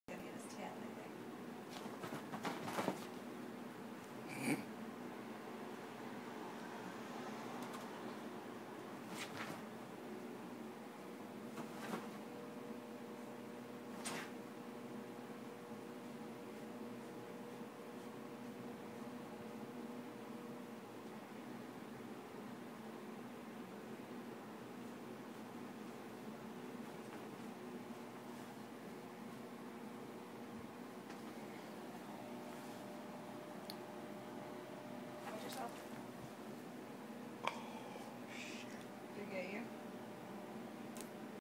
Sam chases his tail
And I deserve the injury I stupidly inflict on myself.